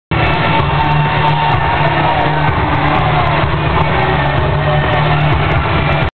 Badd Blood Barron and Kaito vs White Boy and Halloween Pt 2
Bruiser Wrestling May 17th, 2008 in Auburn, Indiana. Badd Blood
and Kaito fight for the tag titles. Part 2 of 3